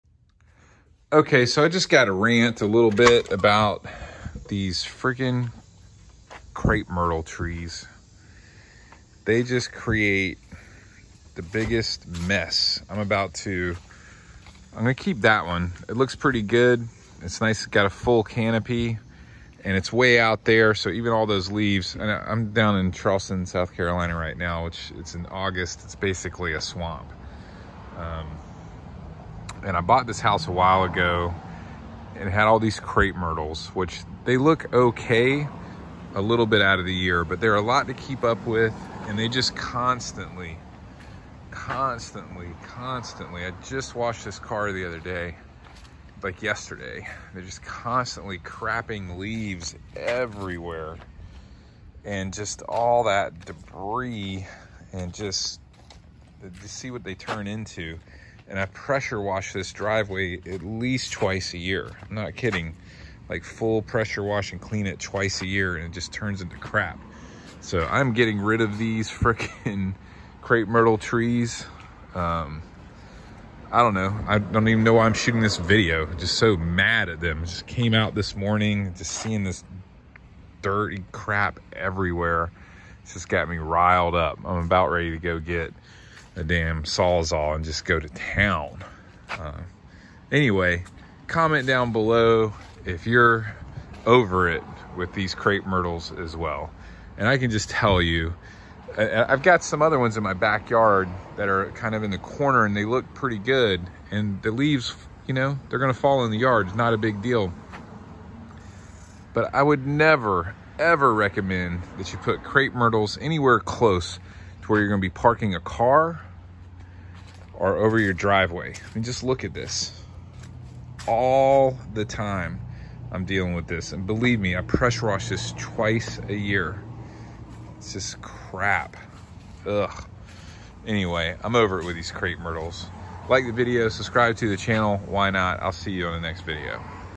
I am over it with these Crape Myrtle Trees
I like the way Crape Myrtle Trees look and I love nature and all of that but these Crape Myrtle Trees along my driveway are really messing up my car and simply must go, don't be mad at me.  I was actually able to save many of them and gave them away free to people that wanted them.